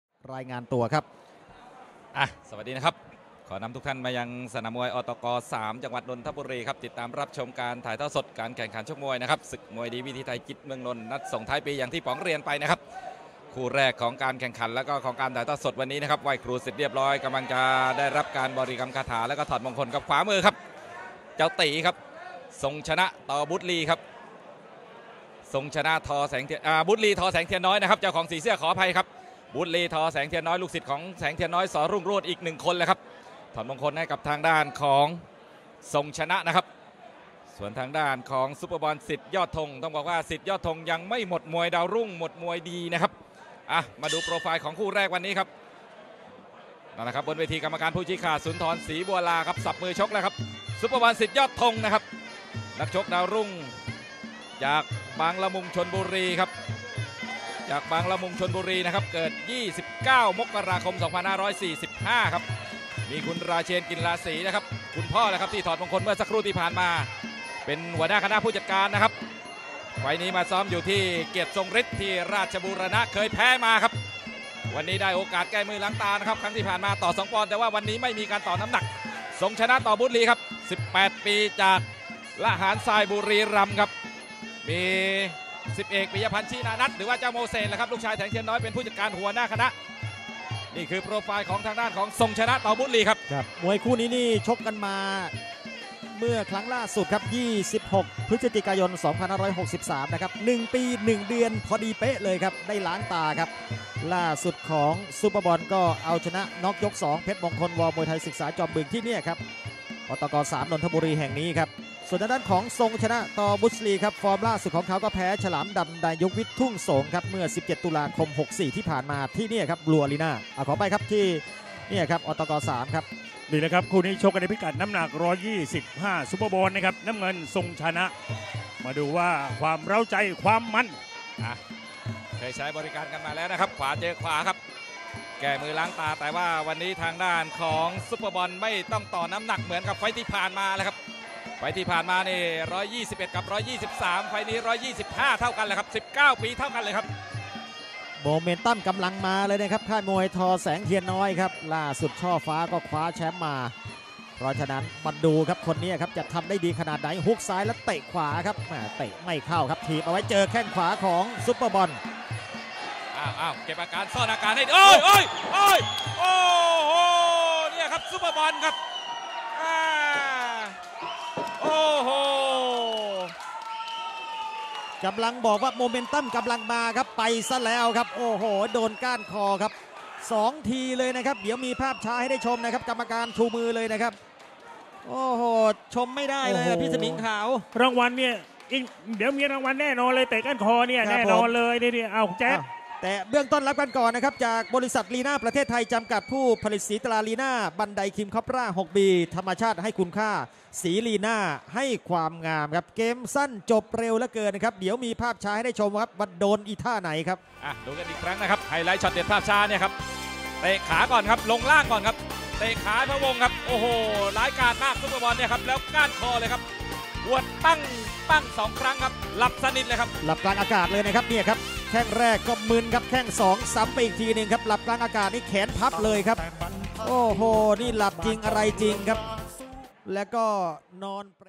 YOKKAO Muay Thai KO: Superball Sityodtong vs Songchana Tor.BruceLee | Aor Tor Gor Stadium by YOKKAO
YOKKAO Muay Thai 125lbs: Superball Sityodtong (red) vs Songchana Tor.BruceLee (blue) | Aor Tor Gor Stadium by YOKKAO. Dec 26th 2021.